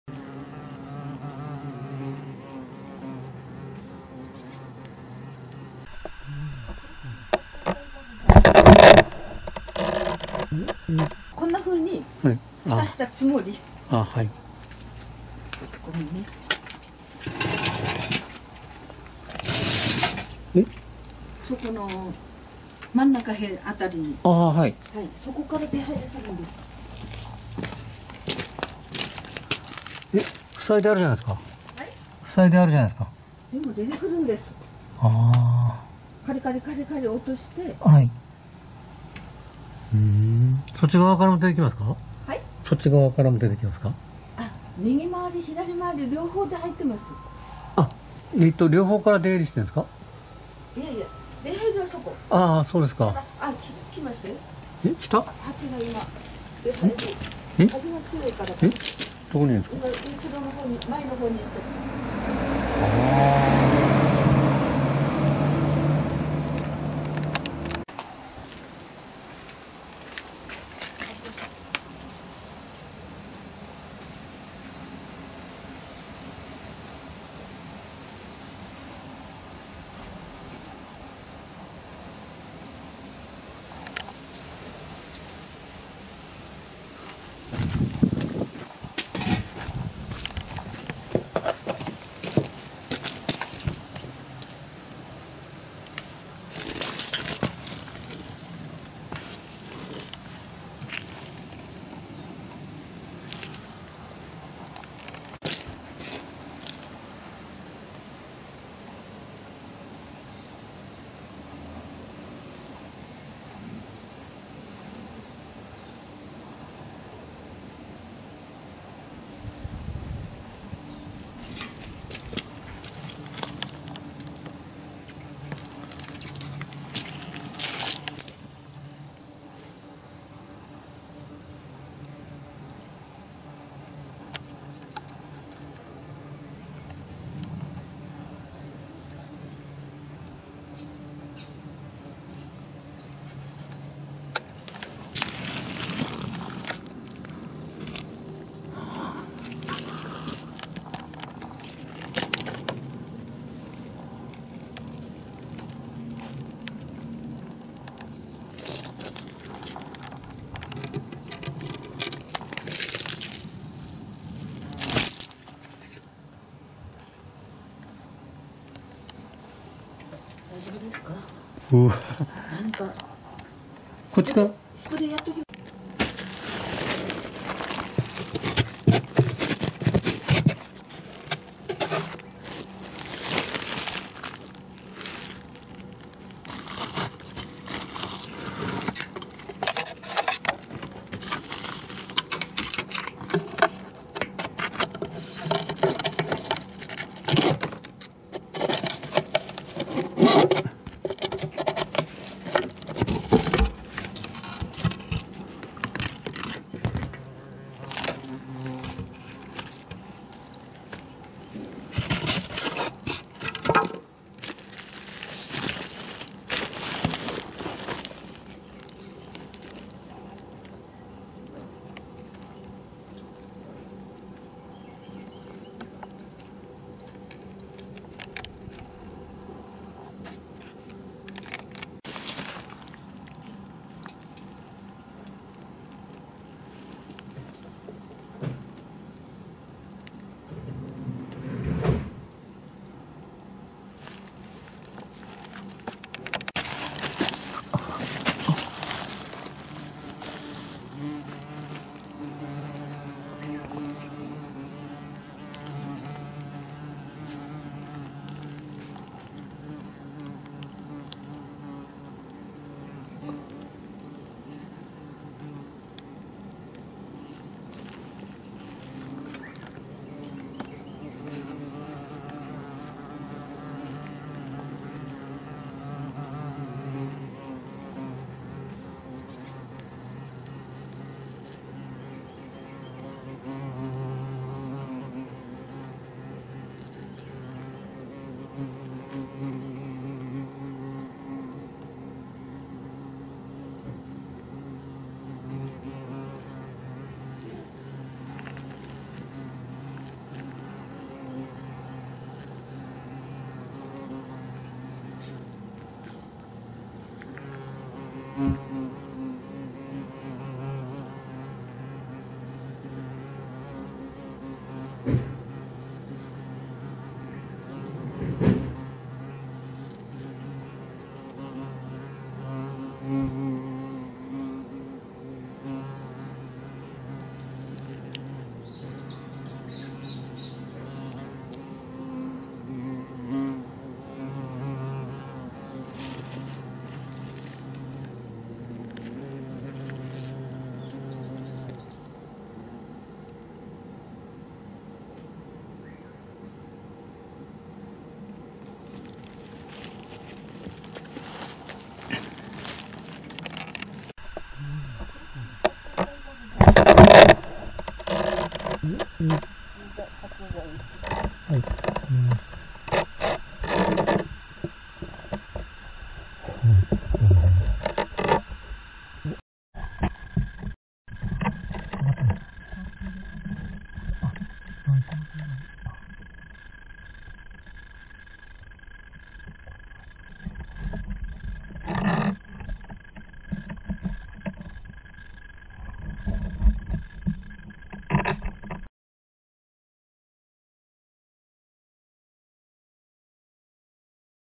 三春町でハチ駆除・スズメバチ駆除- スズメバチが通気口から出入り！通気口を塞ぐが・・・
三春町で通気口からスズメバチが出入りしていたので、通気口を発砲スチロールで塞ぎました。
ところが、スズメバチの通気口からの出入りは止まりません（２０２０年７月１日）。
スズメバチは、キイロスズメバチ（Vespa simillima）です。

キイロスズメバチのことを もっと詳しく知りたい方はこちらへアクセスしてみてください。

○キイロスズメバチが よく巣を作る場所

ルイワン 蜂・害虫駆除センターでは、福島県内のスズメバチ、アシナガバチやミツバチなど蜂の巣駆除のご依頼、ご相談を承っています。